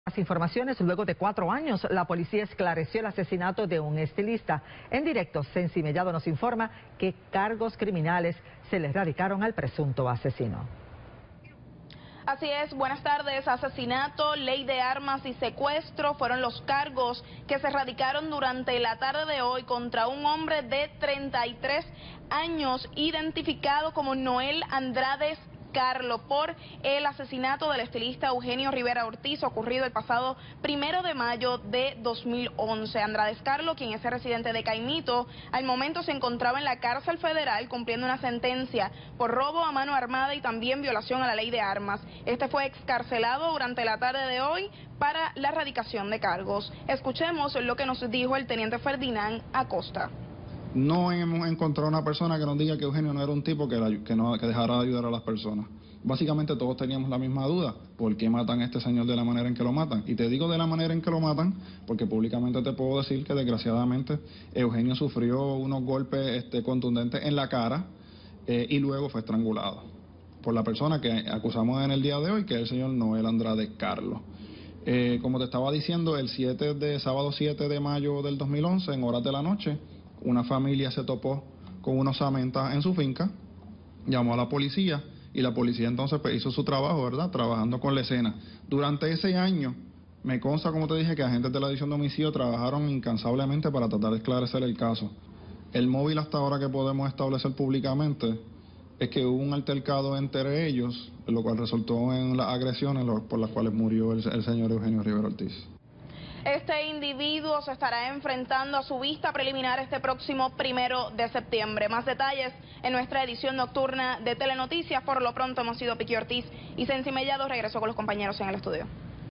Acusado de asesinar estilista en el 2011
Luego de cuatro aÃ±os la PolicÃ­a esclareciÃ³ el asesinato del estilista, Eugenio Rivera Ortiz, el pasado 1ro de mayo de 2011.

asesinato,acusados,Crimen,Telenoticias,Policia,Estilista,telemundo